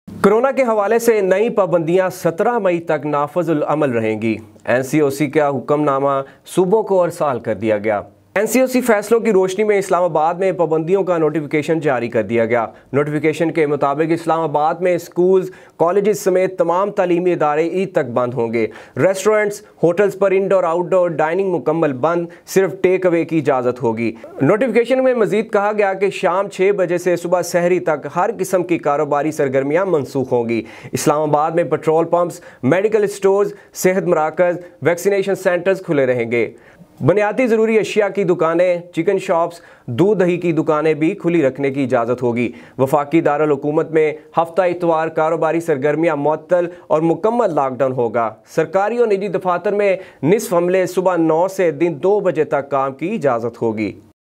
کورونا کا پھیلاؤ روکنے کے لیے نئی پابندیاں
کرونا کے حوالے سے نئی پابندیاں 17 مئی تک نافذ